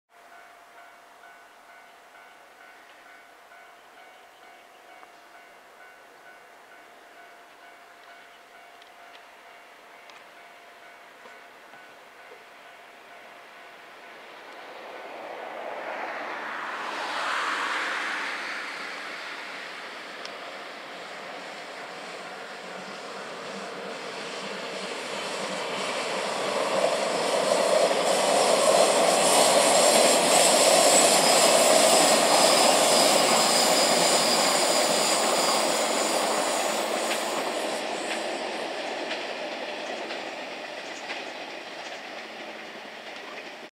583系 わくわくドリーム号 2015 0619
583系 わくわくドリーム号
6月分最終 舞浜行き
夏至間近、夕闇せまる秋田路を走行。
奥羽本線 鶴形〜東能代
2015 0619